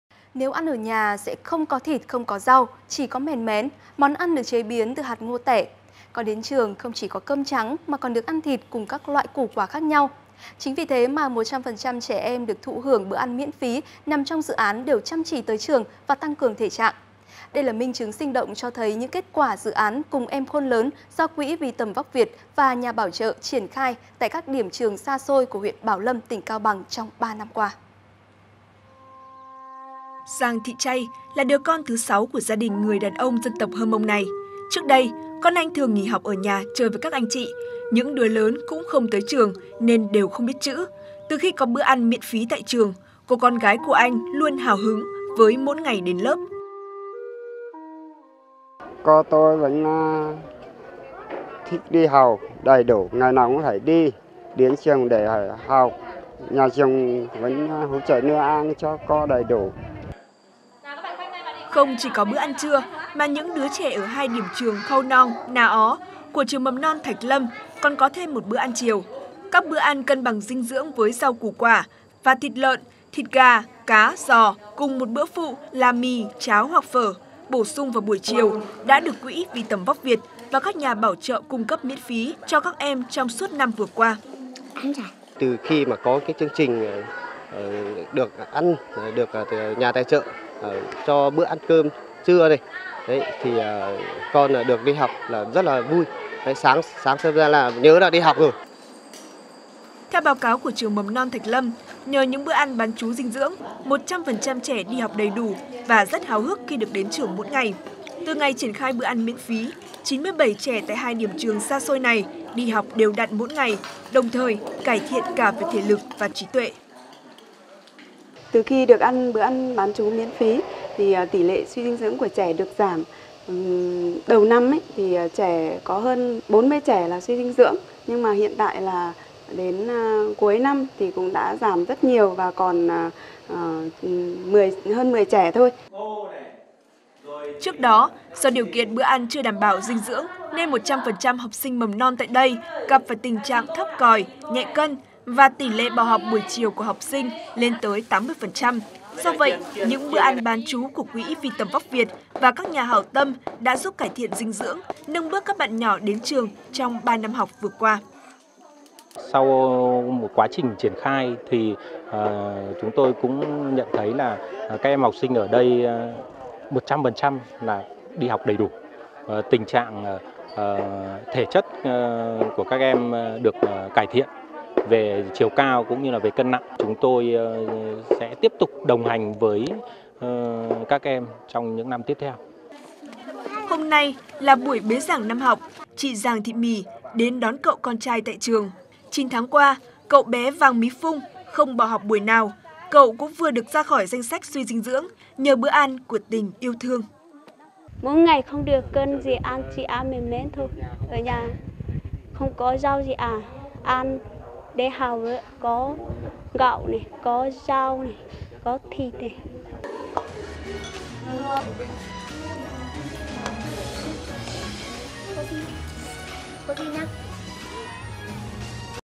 Những bữa ăn bán trú miễn phí từ dự án "Cùng em khôn lớn" cho học sinh mầm non tại Cao Bằng
Nếu ăn ở nhà sẽ không có thịt, không có rau, chỉ có mèn mén – món ăn được chế biến từ hạt ngô tẻ; còn đến trường không chỉ cơm trắng mà còn được ăn thịt lợn cùng các loại củ quả khác nhau. Chính vì thế mà 100% trẻ được thụ hưởng bữa ăn miễn phí nằm trong đều chăm chỉ tới trường và tăng cường thể trạng. Đây là minh chứng sinh động cho thấy những kết quả Dự án “Cùng em khôn lớn” do Quỹ Vì Tầm Vóc Việt và nhà bảo trợ triển khai tại các điểm trường xa xôi của huyện Bảo Lâm, tỉnh Cao Bằng trong 3 năm qua.

Giàng Thị Chay là đứa con thứ 6 của gia đình Giàng Giống Sinh người dân tộc Mông. Anh chia sẻ trước đây, con anh thường nghỉ học ở nhà chơi với các anh chị, những đứa lớn cũng không tới trường nên đều không biết chữ. Từ khi có bữa ăn miễn phí tại trường, cô con gái của anh luôn hào hứng với mỗi ngày đến lớp.

Theo báo cáo của trường Mầm non Thạch Lâm, nhờ có những bữa ăn bán trú dinh dưỡng, 100% trẻ đi học đầy đủ, và rất háo hức được đến trường mỗi ngày. Từ ngày triển khai bữa ăn miễn phí, 97 trẻ tại hai điểm trường xa xôi này đi học đều đặn mỗi ngày, đồng thời được cải thiện cả về thể lực và trí tuệ. Bà Nguyễn Thị Liêm - Hiệu trưởng Trường Mầm non Thạch Lâm, huyện Bảo Lâm, tỉnh Cao Bằng cho biết.

Trước đó, do điều kiện bữa ăn chưa được đảm bảo dinh dưỡng, nên 100% học sinh mầm non tại đây gặp phải tình trạng thấp còi, nhẹ cân và tỉ lệ bỏ học buổi chiều của học sinh lên tới 80%. Đặc biệt, điểm trường Khau Noong chưa có điện nên việc học tập của các em gặp rất nhiều khó khăn.

Ông Vũ Xuân Thái - Quản lý chương trình Quỹ Vì Tầm Vóc Việt thông tin thêm: “Sau 1 quá trình triển khai chúng tôi nhận thấy các em học sinh đi học đầy đủ, tình trạng thể chất của các em được cải thiện về chiều cao và cân nặng chúng tôi sẽ tiếp tục đồng hành cùng các em trong những năm tiếp theo…”

😍Trân trọng cảm ơn Đài truyền hình kỹ thuật số VTC đã đồng hành và đưa tin về chuyến đi của Quỹ Vì Tầm Vóc Việt!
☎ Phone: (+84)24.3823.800